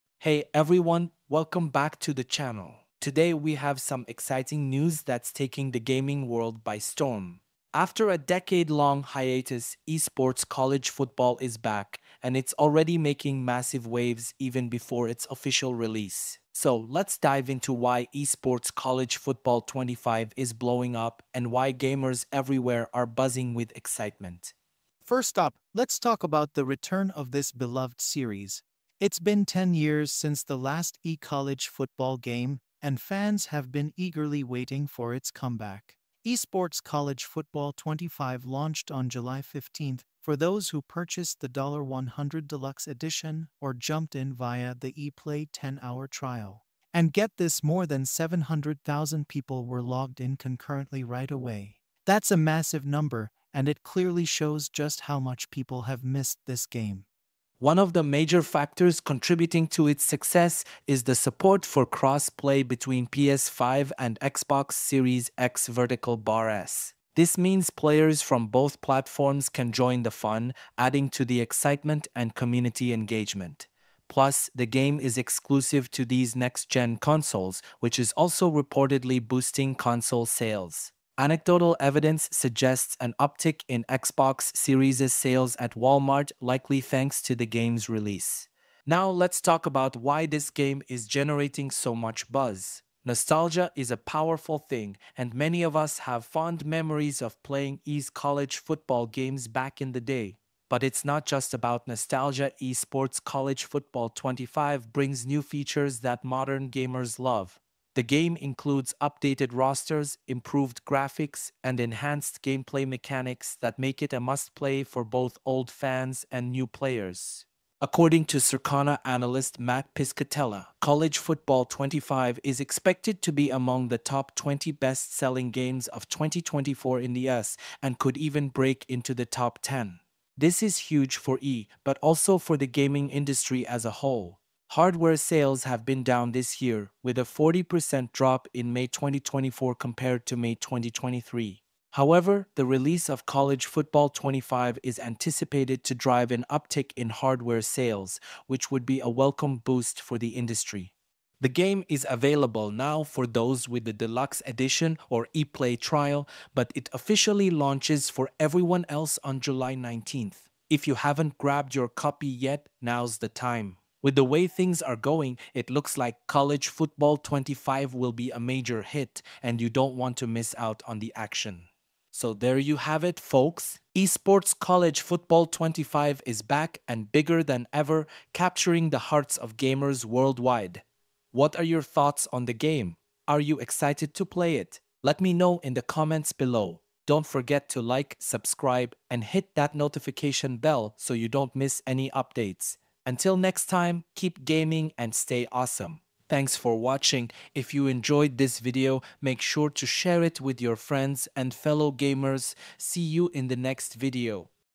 Why This Game Is Already a Massive Hit Before Its Official Release!
In this video, we dive into why EA Sports College Football 25 is already a massive hit even before its official release! 🌟 After a decade-long hiatus, the beloved college football game is back, and it’s creating a huge buzz among gamers. With over 700,000 concurrent players logging in early, this game is setting the stage for an epic comeback. 🎮

We’ll explore the nostalgia factor, the new features, and why cross-play between PS5 and Xbox Series X|S is a game-changer. Plus, we’ll look at the industry impact and predictions for this game’s success in 2024. Whether you're a long-time fan or new to the series, this video has everything you need to know about EA Sports College Football 25!